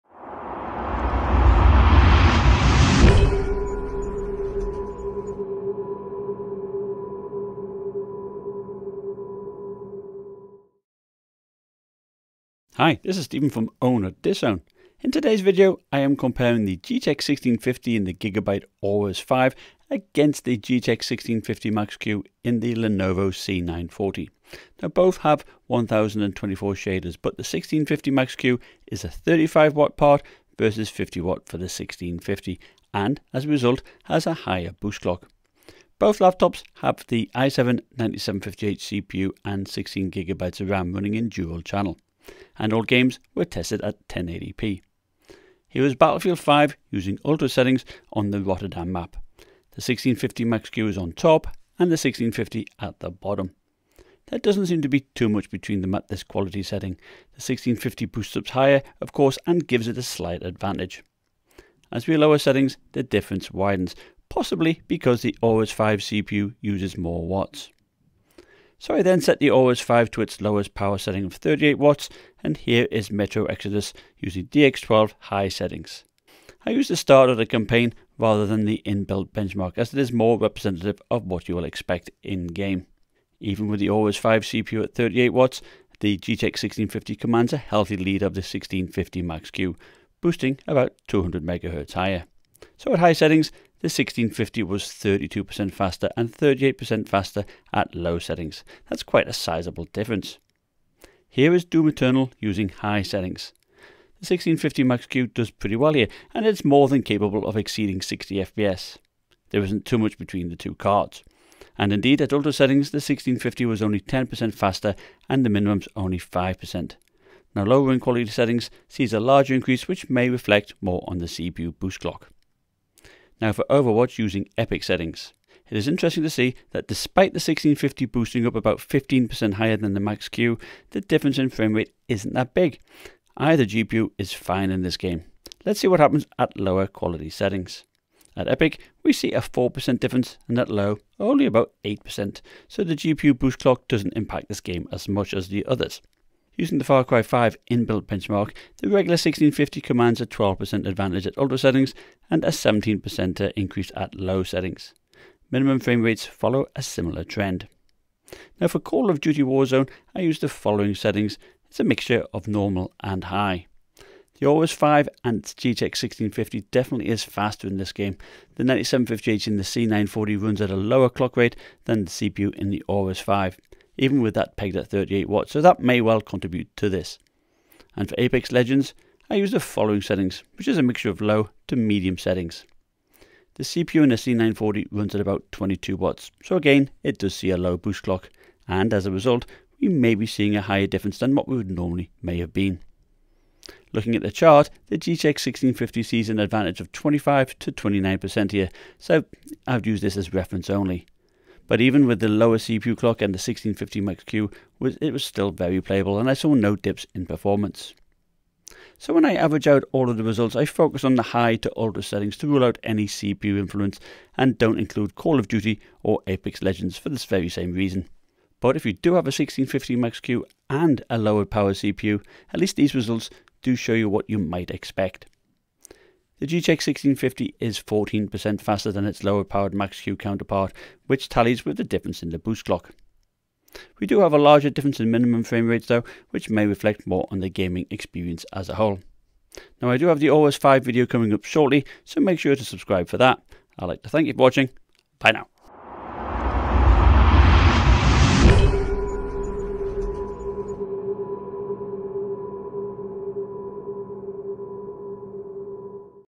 GTX 1650 vs GTX 1650 Max Q - Budget GPU Comparison
Get high-quality skins & wraps for your laptops, phones, and handhelds here 👉 m2skins.com The GTX 1650 and GTX 1650 Max Q GPUs are found in budget gaming and thin and light 15" laptops. In this video I compare the gaming performance difference.
Units used are:
Aorus 5
i7 9750h
GTX 1650
2 x 8GB RAM

Lenovo Yoga C940
i7 9750h
GTX 1650 MQ
2 x 8GB RAM